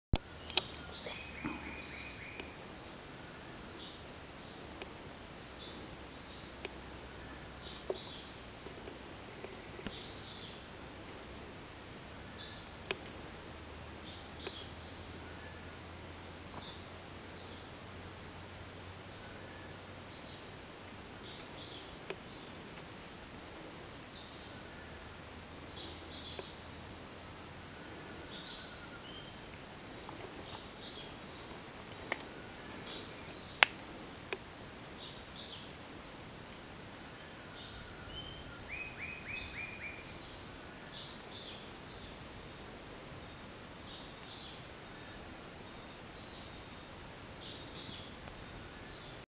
Snake eating frog
A snake eating a frog in the Barra Honda National Park of Costa Rica